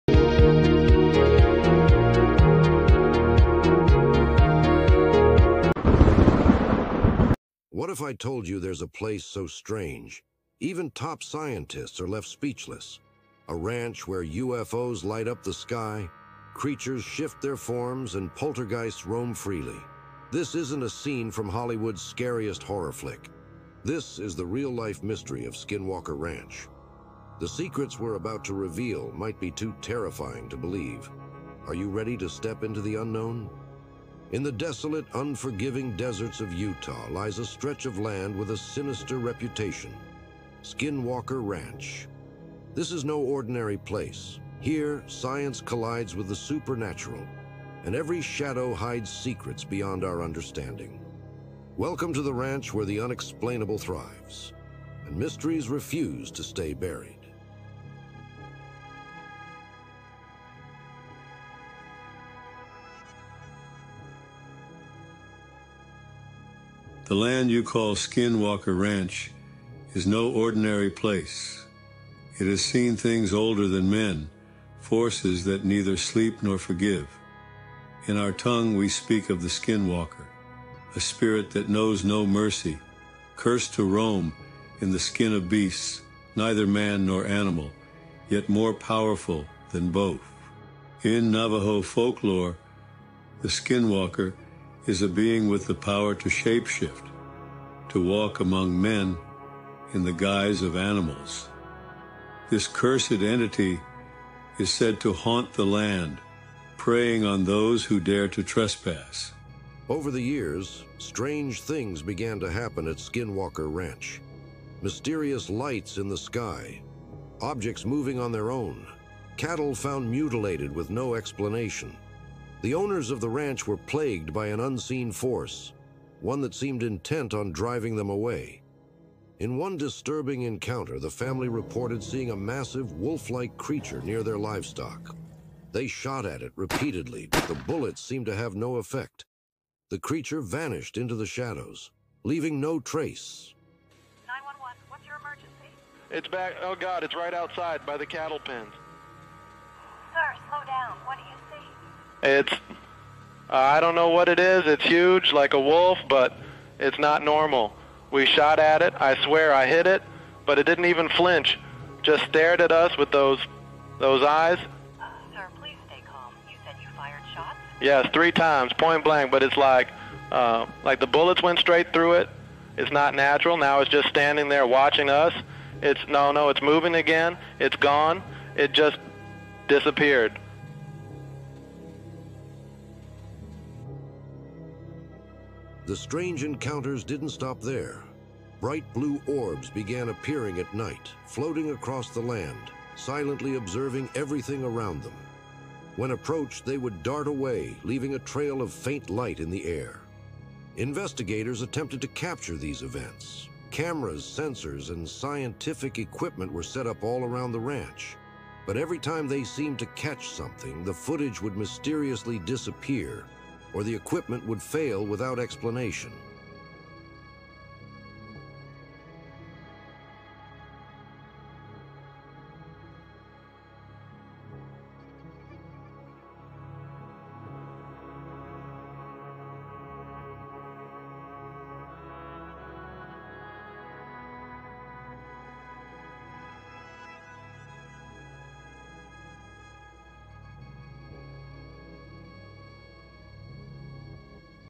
Skinwalker Ranch: UFOs, Shapeshifters, and Chilling Paranormal Secrets-Episode 2
Explore the terrifying mysteries of Skinwalker Ranch in this AI-driven faceless horror video. Uncover UFO sightings, shapeshifters, and poltergeist activity in one of the world's most haunted locations!"